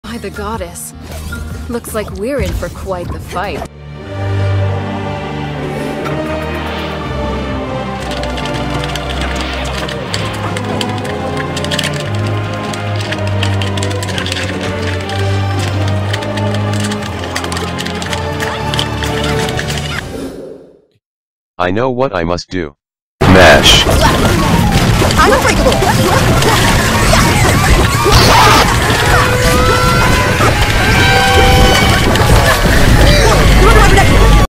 Steven's SACRED MASH Perk
Since when did Steven players unlock the secret mash perk. Because this was the MASHIEST Steven in the entire UNIVERSE (lol)
➤ Main channel: @AmariDT